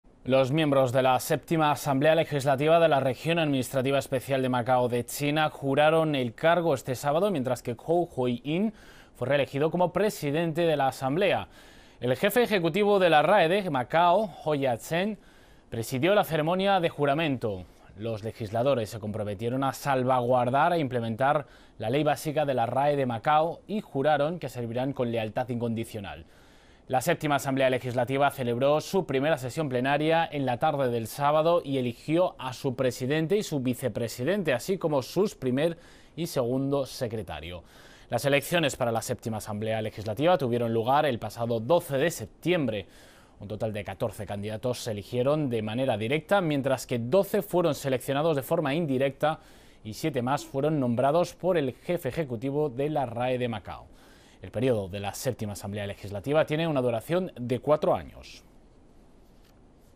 Los miembros de la VII Asamblea Legislativa de la RAE de Macao juran el cargo
Los miembros de la séptima Asamblea Legislativa de la Región Administrativa Especial de Macao de China juraron el cargo este sábado, mientras que Kou Hoi In fue reelegido como presidente de la Asamblea. El jefe ejecutivo de la RAE de Macao, Ho Iat Seng, presidió la ceremonia de juramento. Los legisladores se comprometieron a salvaguardar e implementar la Ley Básica de la RAE de Macao, y juraron que servirán con lealtad incondicional. La séptima Asamblea Legislativa celebró su primera sesión plenaria en la tarde del sábado, y eligió a su presidente y su vicepresidente, así como sus primer y segundo secretario. Las elecciones para la séptima Asamblea Legislativa tuvieron lugar el pasado 12 de septiembre. Un total de 14 candidatos se eligieron de manera directa, mientras que 12 fueron seleccionados de forma indirecta y siete más fueron nombrados por el jefe ejecutivo de la RAE de Macao. El periodo de la séptima Asamblea Legislativa tiene una duración de cuatro años.